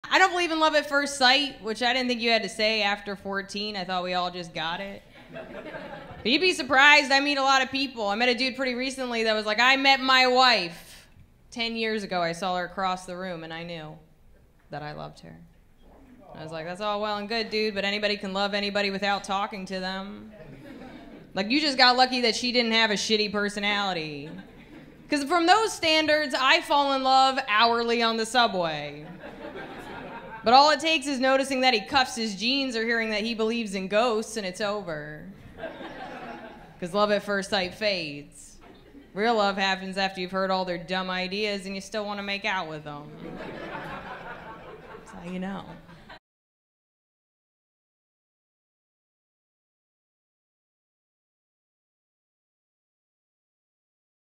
LOVE AT FIRST SIGHT ISN'T REAL - Liz Miele
Spewing real talk in my 20s. My favorite part of this joke is my new hour has a 5 min joke about ghosts not being real. So I guess I haven't changed much. I think this is from my first hour "Emotionally Exhausting" which is free on here.
Come see me this month in RIchmond, Chicago, Kansas City and St. Louis.